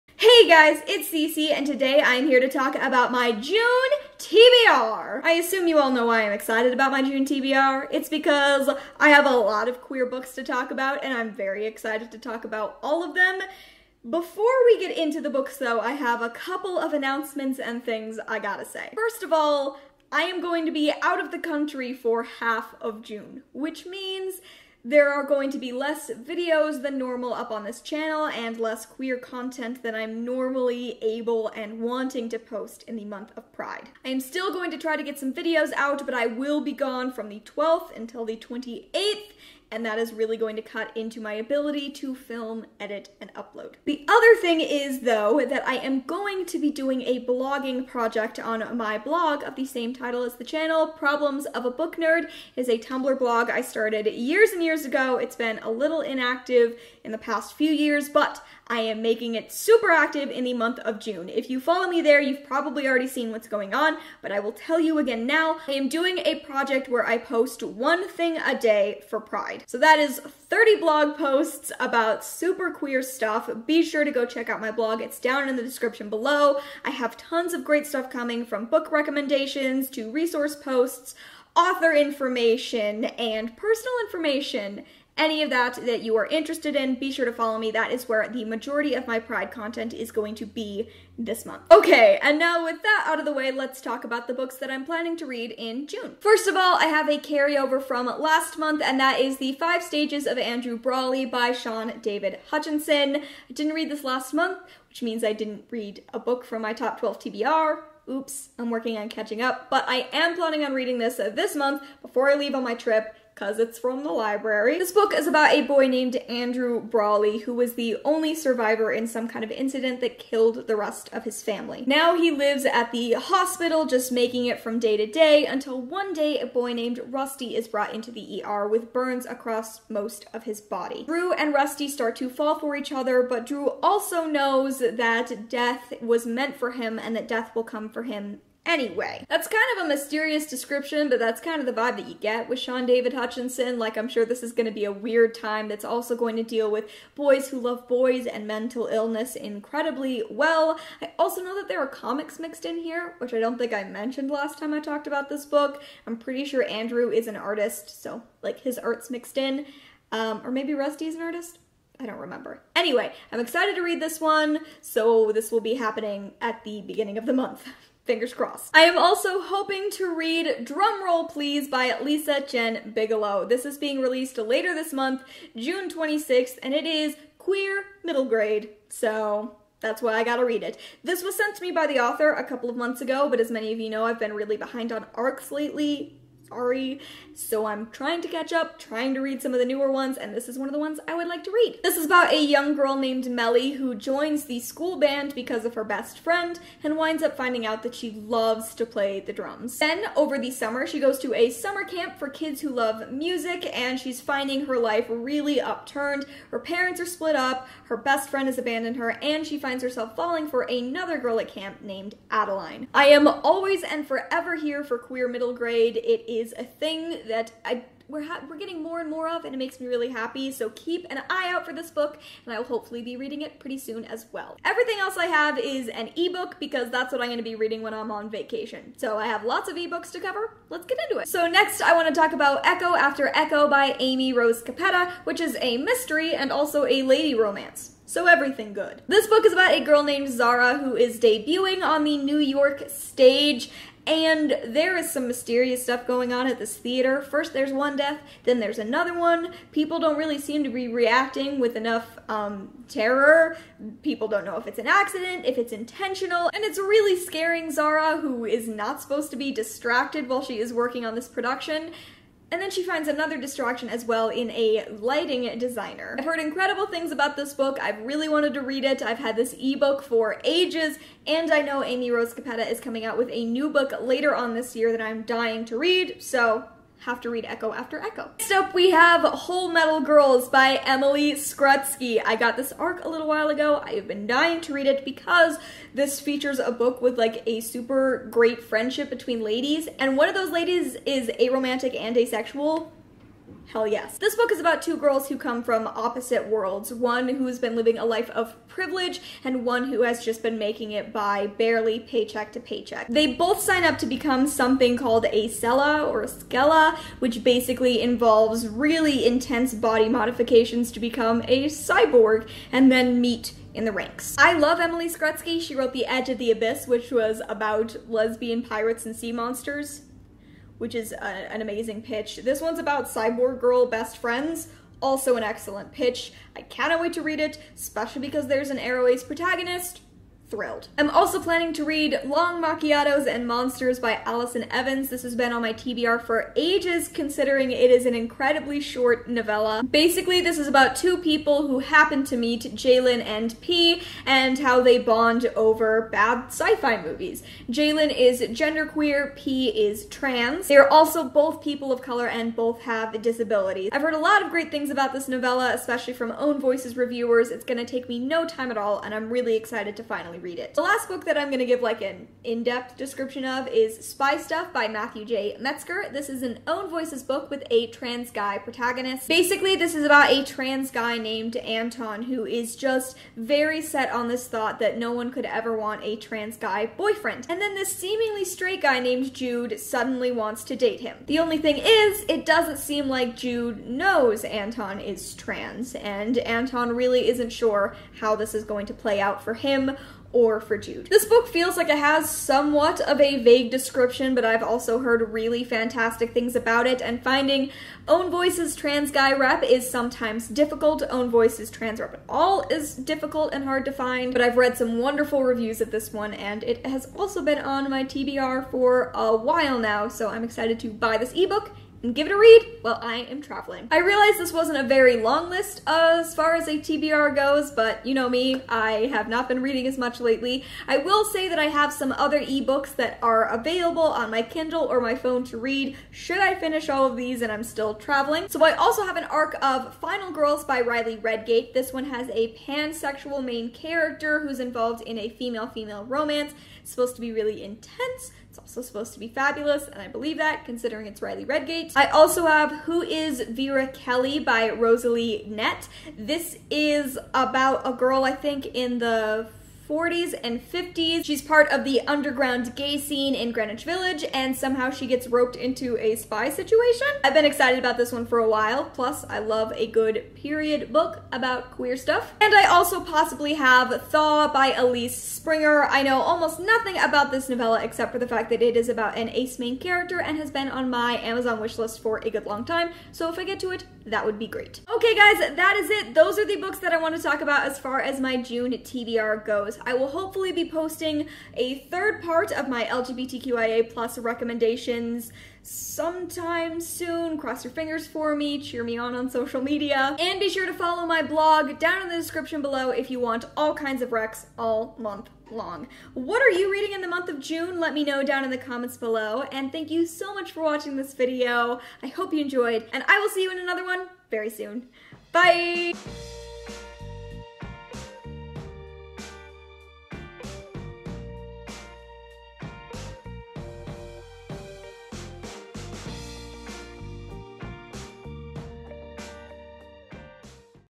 JUNE TBR | 2018
I have lots of queer books to read this Pride!

Appreciate the content I make?

Books mentioned:
The Five Stages of Andrew Brawley by Shaun David Hutchinson
Drum Roll Please by Lisa Jenn Bigelow
Echo After Echo by Amy Rose Capetta
Hullmetal Girls by Emily Skrutskie
Long Macchiatos and Monsters by Alison Evans
Spy Stuff by Matthew J. Metzger
Final Draft by Riley Redgate
Who Is Very Kelly? by Rosalie Knect
Thaw by Elyse Springer